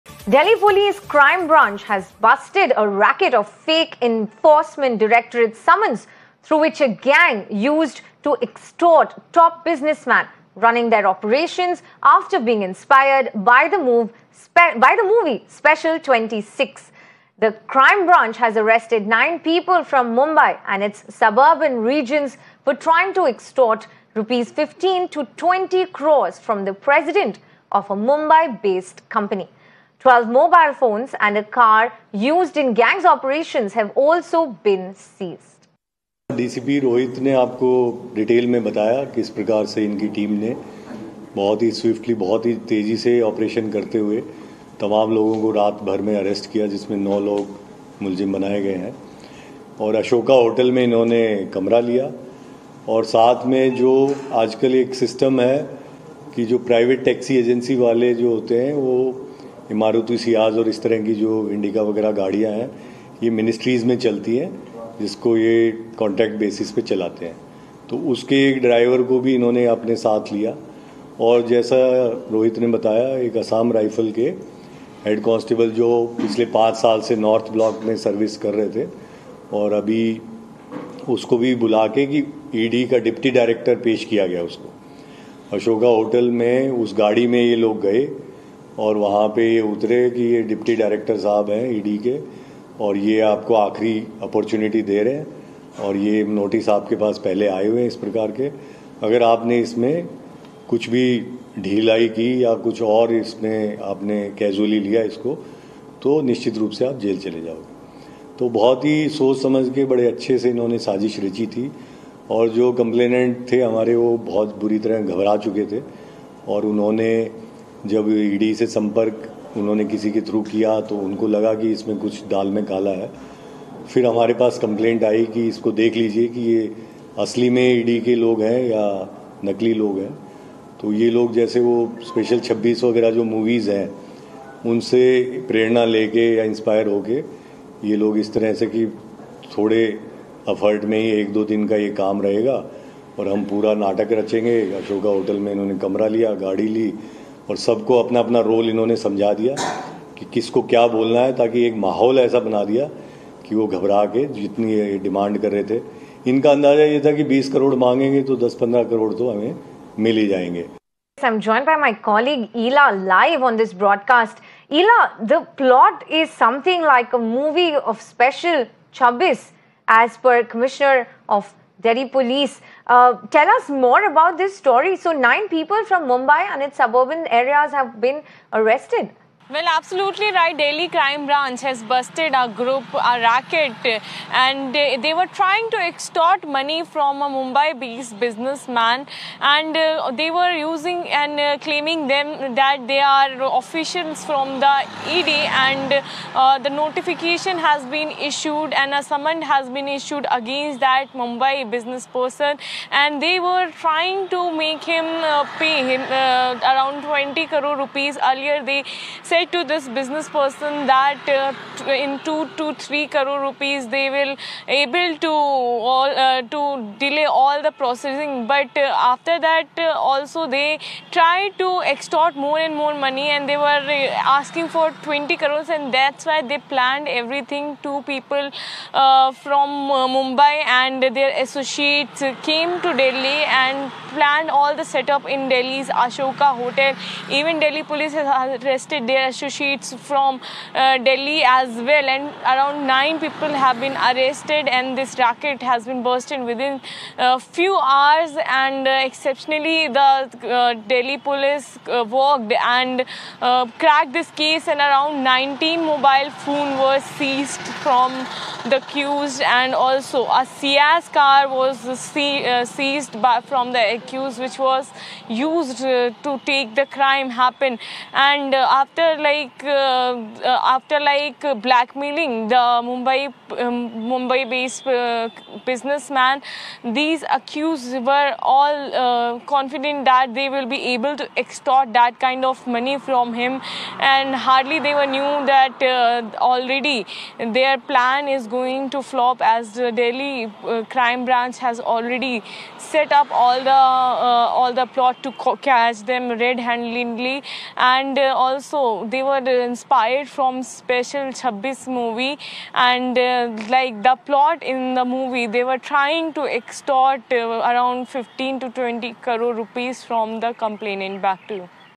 Fake ED summons racket busted in Delhi; gang inspired by bollywood movie 'Special 26'
Crime branch of Delhi Police has busted a fake ED summons racket. This multi-crore extortion ring bust has led to the arrests of 9 people. The gang ran their ops after being inspired by the movie Special 26.